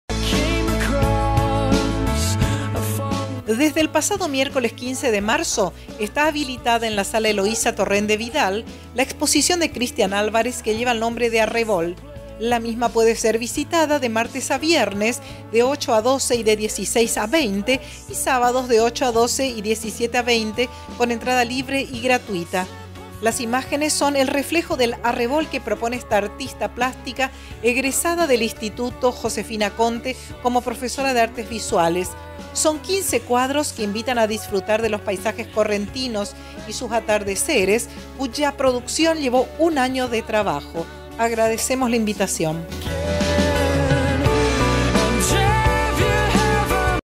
Crhistian Alvarez
En el Museo de Bellas Artes se encientra exponiendo la artísta plástica Christian Alvarez . . .